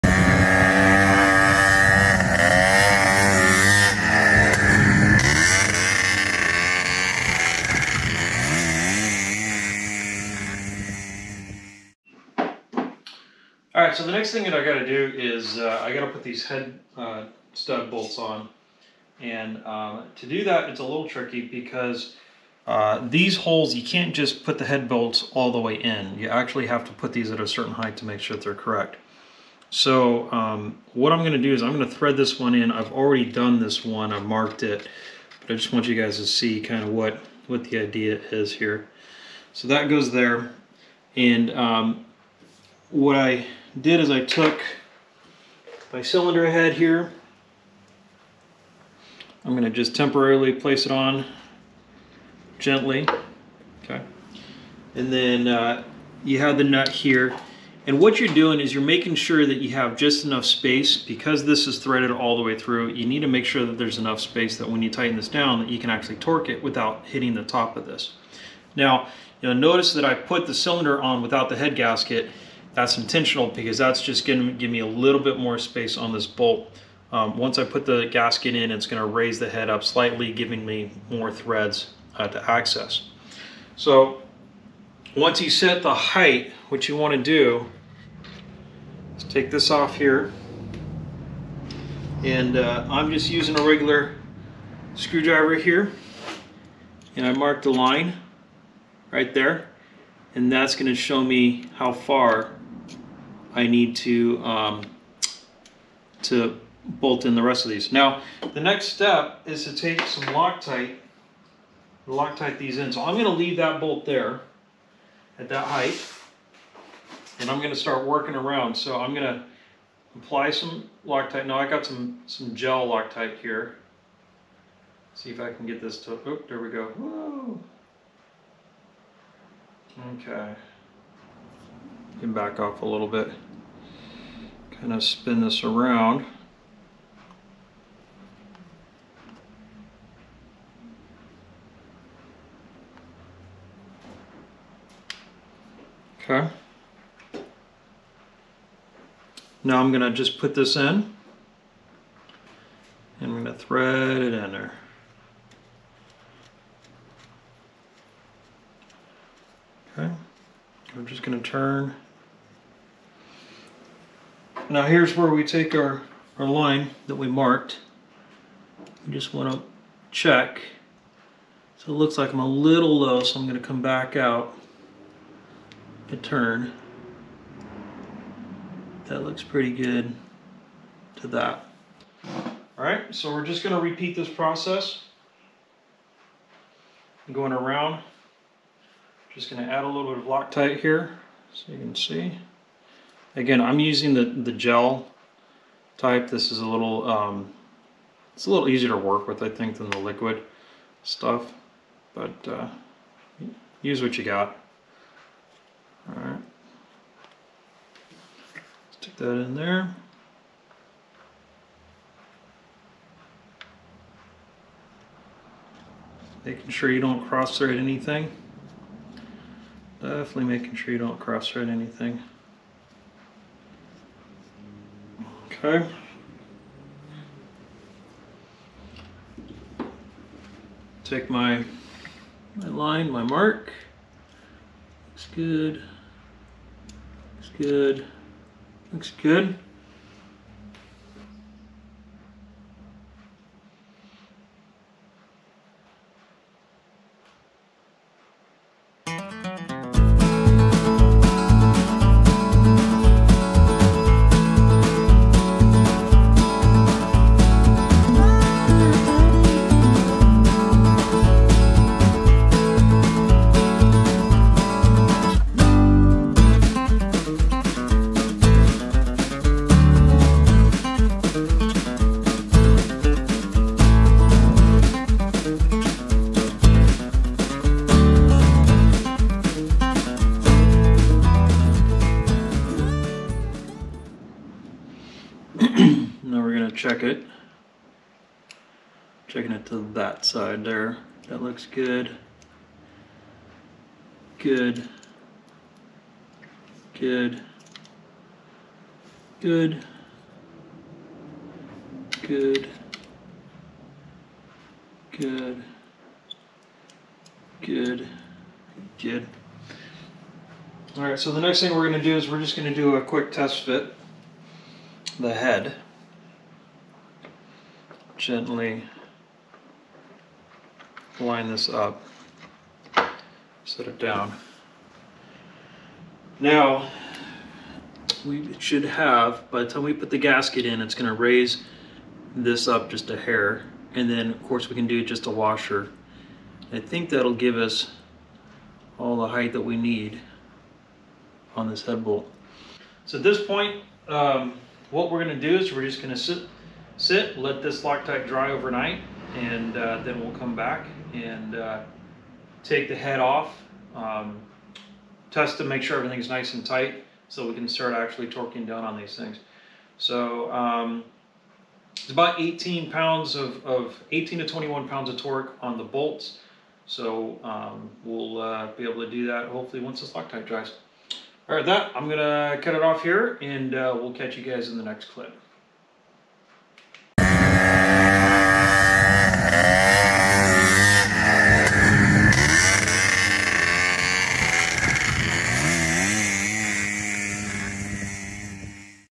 Honda Odyssey FL350 Engine Rebuild Part 11 - Water Cooled Head Stud Install
In this brief video I continue the FL350 engine rebuild by installing the head bolt studs onto my engine cylinder. The reason I am doing this is because I am changing to a new water cooled head that requires changing out the head  stud bolts.